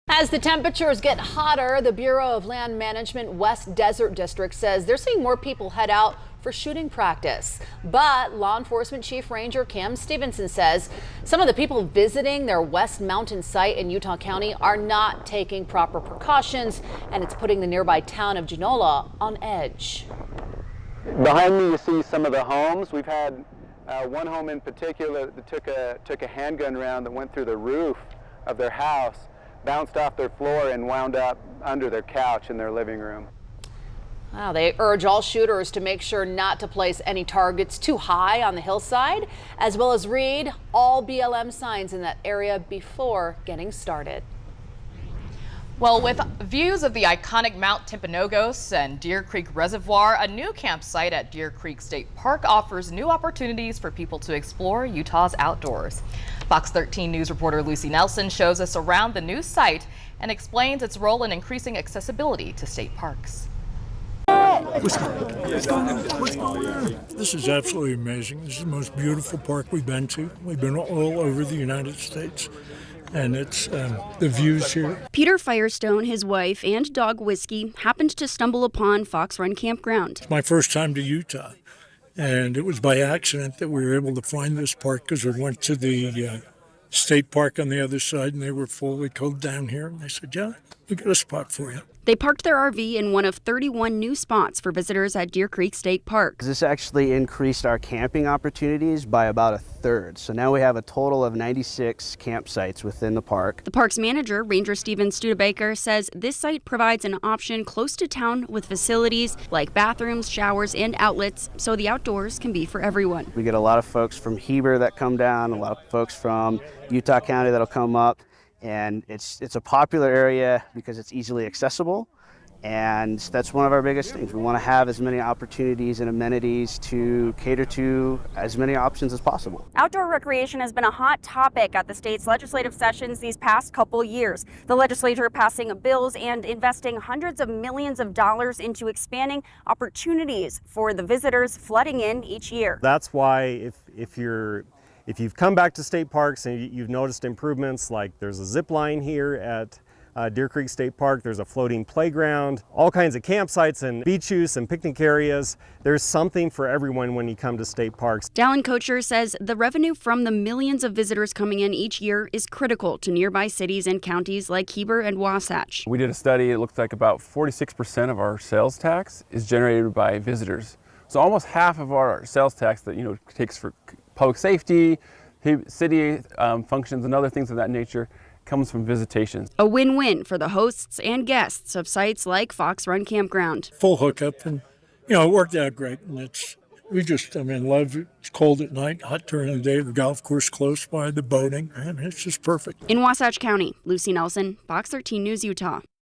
'Absolutely amazing!;' Visitors praise new campsites at popular Utah state park
With views of the iconic Mount Timpanogos and the Deer Creek Reservoir, a new campsite at Deer Creek State Park offers new opportunities for people to explore Utah’s outdoors.